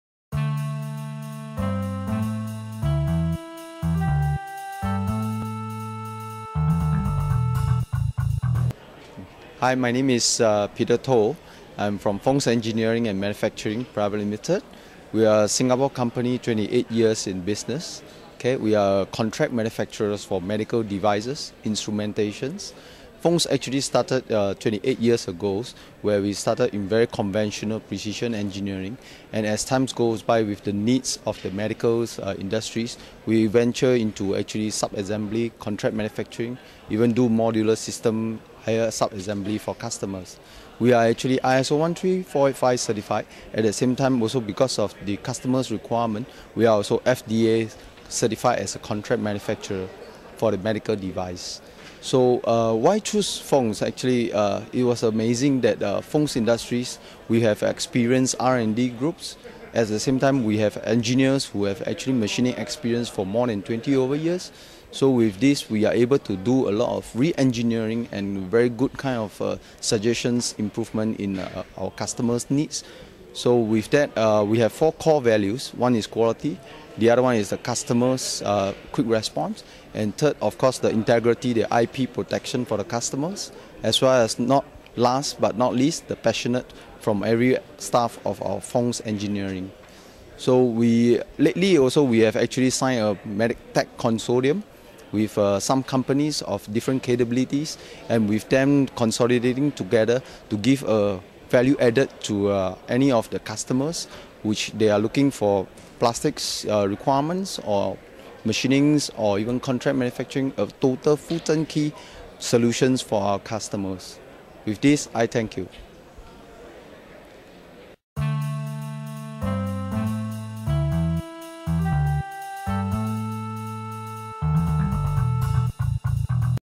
Fong's Peter Toh explains contract manufacturing for medical device OEMs
Peter Toh K. H., Senior Manager of Sales/Programme Department at Fongs Engineering & Manufacturing, explains the companys contract manufacturing capabilities. Additionally, he explains the re-engineering services that enable Fongs engineers to improve on customers products. The company observes four values: quality, quick response, integrity (IP protection), and passion. to find out more about Fongs Engineering & Manufacturing.